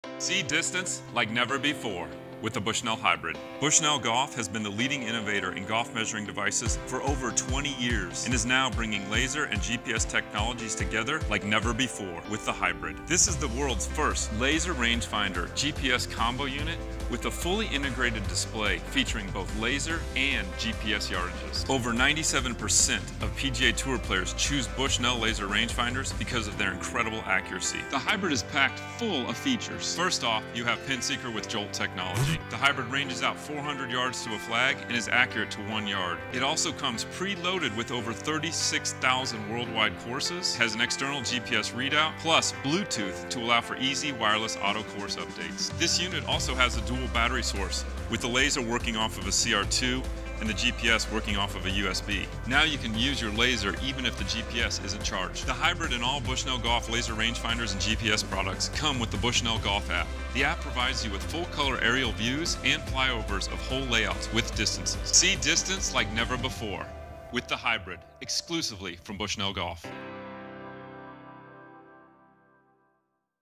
Bushnell Hybrid Laser Rangefinder + GPS
See distance like never before with the Bushnell Hybrid.  Bushnell Golf has been the leading innovator in Golf Measuring Devices for over 20 years and is now bringing Laser and GPS technologies together like never before with the Hybrid.  This is the world’s first Laser Rangefinder/GPS combo unit with a fully integrated display featuring both Laser and GPS yardages.

Over 97% of PGA Tour players choose to use a Bushnell Laser Rangefinder due to their unrivaled precision.  Bushnell has now combined this unrivaled laser precision with GPS-provided “Front” and “Back” distances in the LCD to further remove the guesswork from your game.  You now have every number you would want going in to every green to approach all of your shots with the utmost confidence.